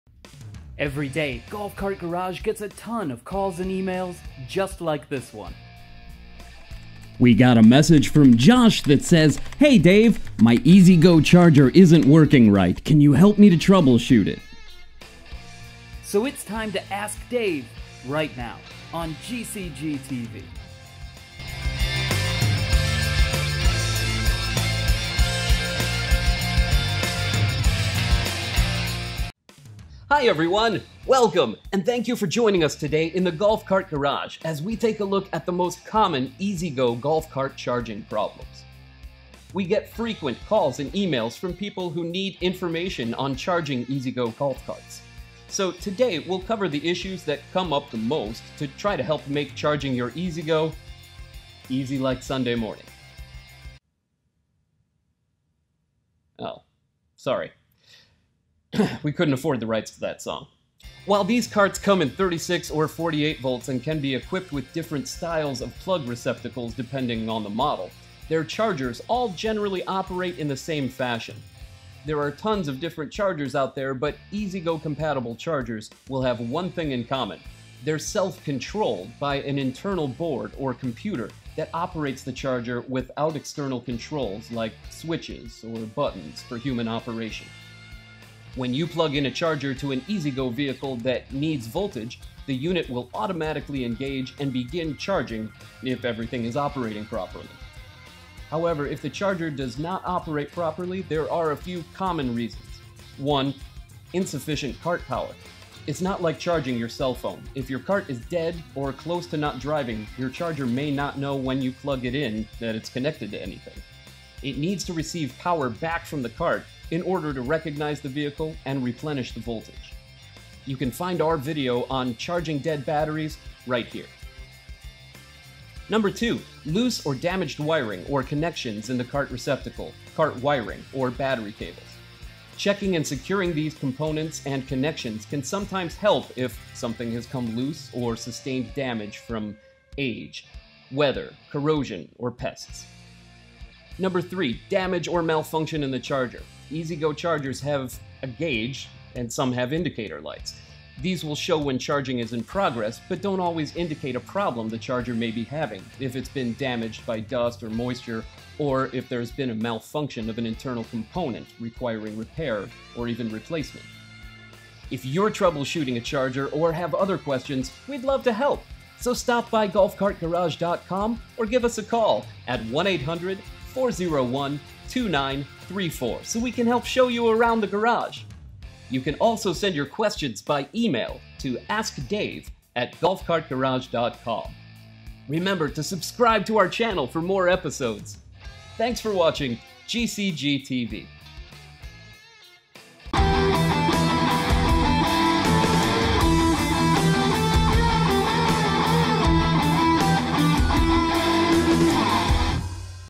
EZGO Charger Troubleshooting | ASK DAVE | Golf Cart Garage
Today on GCGTV, we're covering common issues and troubleshooting chargers for EZGO golf cart drivers after receiving frequent questions from these customers at Golf Cart Garage.

Thanks for stopping by the garage!

0:00 Cold Open
0:10 Today's Question
0:20 Intro Animation
0:30 Overview
1:03 General Info
1:48 Common Issues
2:45 Summary / Contact
3:13 Outro Animation